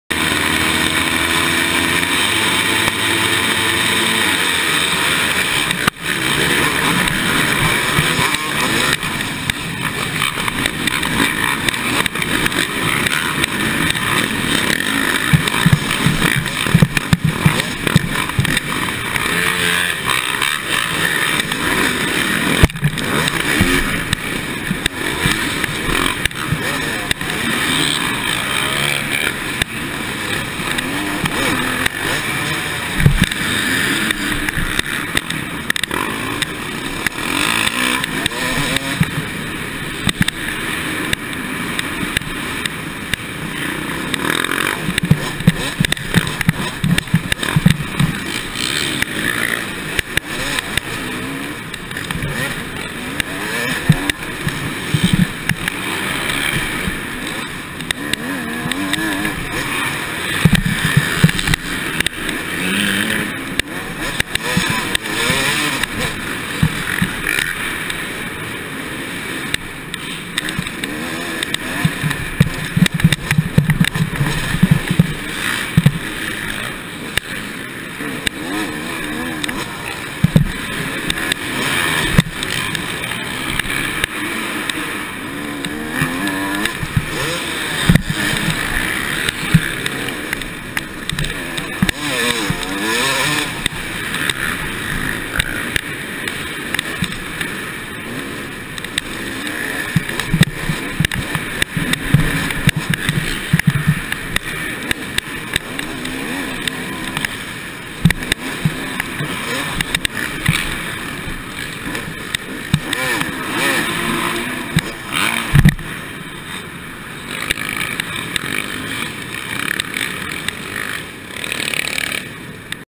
Arenacross 2015 Wilkes Barre Pa C open LCQ
Crashed twice in the first moto leaving me with a second row start. Racing the yz250 2 stroke in the open class got me some fans.